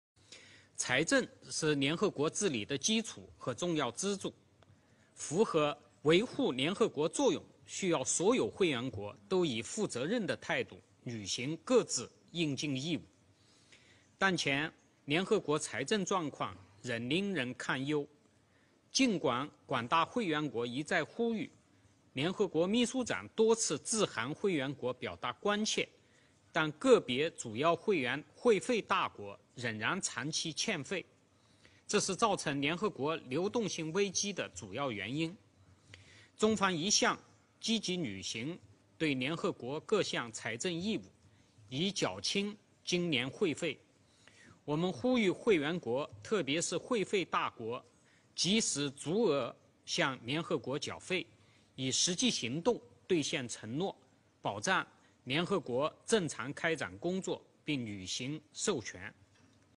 联合国个别主要会费大国长期欠费中方呼吁会员国及时足额缴费
联合国个别主要会费大国长期欠费 中方呼吁会员国及时足额缴费China calls on Member States to pay their contributions in full and on time